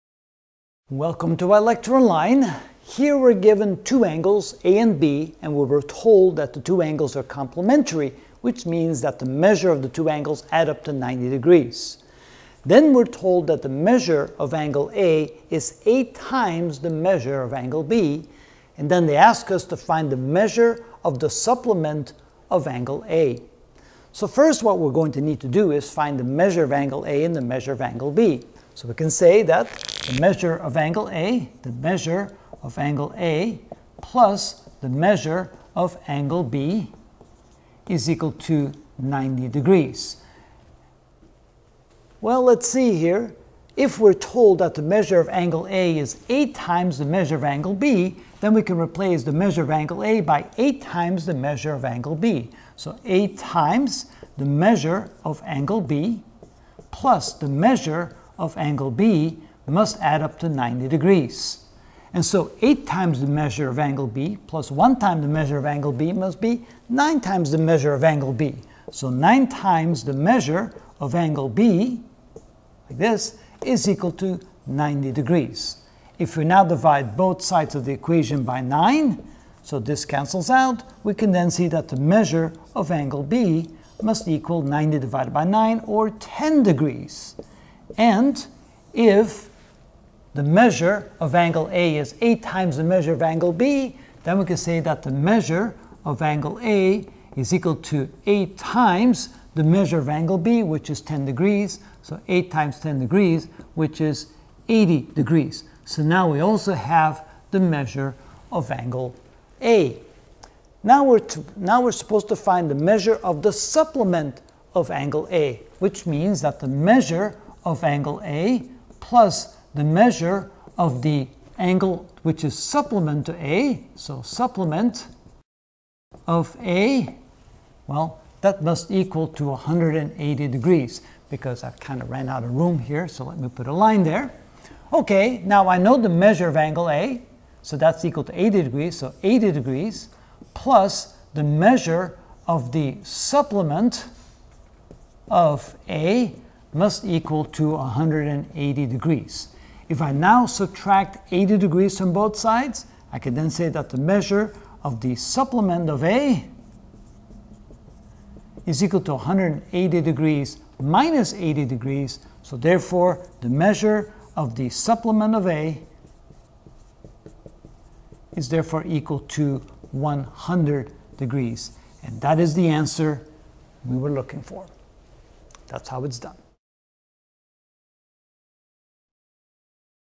Geometry - Ch. 1: Basic Concepts (48 of 49) Review Problem 14: Complimentary Angles
In this video I will find the measure of the supplement of angleA given angleA and angleB are complimentary and measure of angleA is 8 times of measure of angleB.